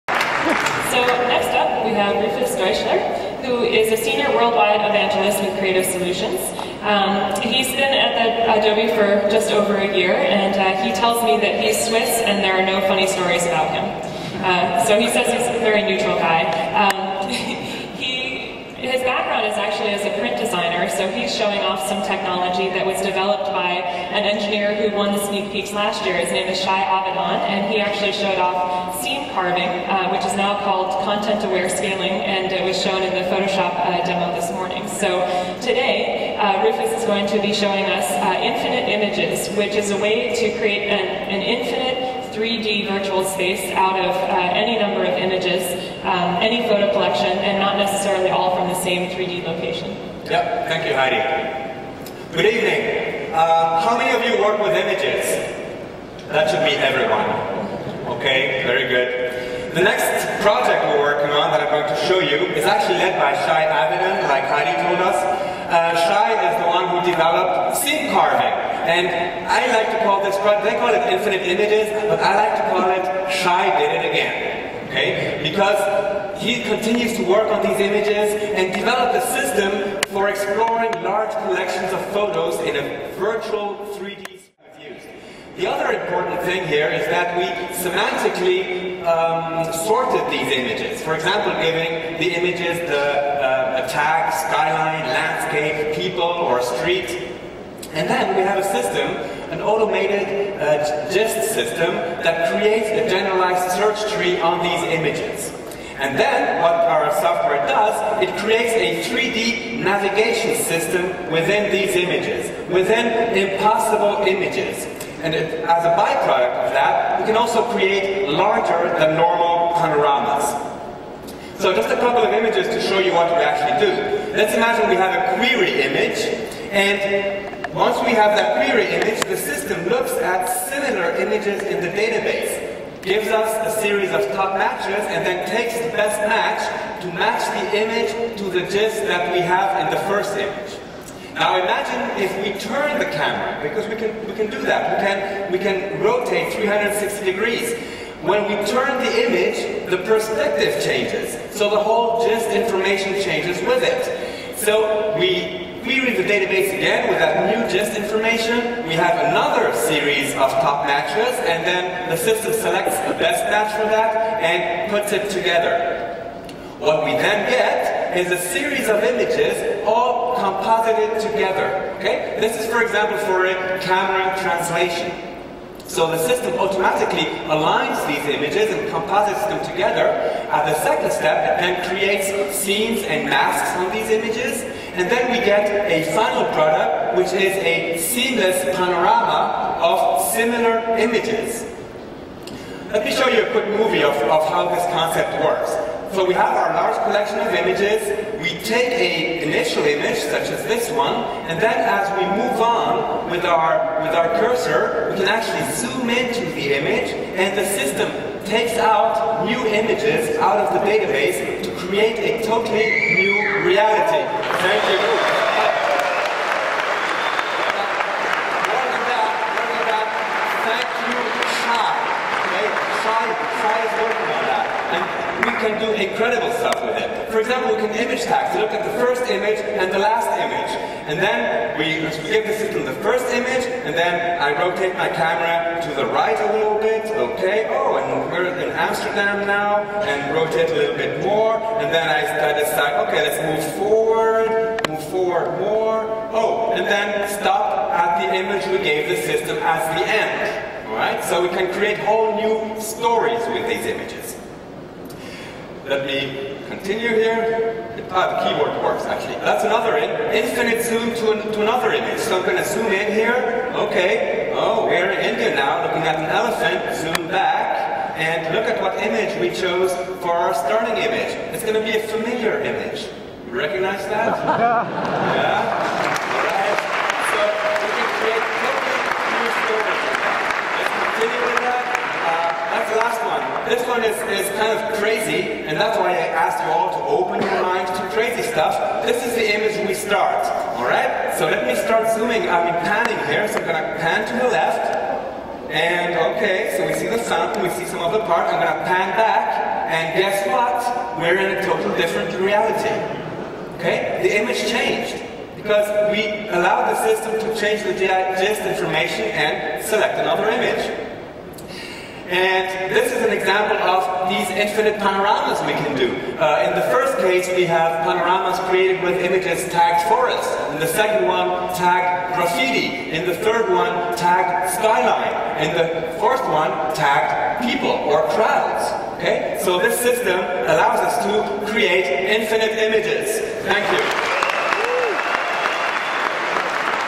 Sneak Peeks - Adobe MAX 2008 Milan - Shai Avidan's Infinite
I had the great privilege to present a technology that "may or may not be part of any Adobe solution in the future" at Adobe MAX 2008 in Milan. This specific project is called "Infinite Images" and is being worked on by Shai Avidan; yes, the one that brought us Seam Carving (better known as Content Aware Scaling) in Adobe Photoshop CS4. The sneak won the applaudimeter test from the audience. Kudos Shai!
Note: thanks to Peter Elst for filming it and sharing this with me (there are about 20 seconds missing in the intro due to a change of batteries). For the full length video of the Milan Sneak Peeks go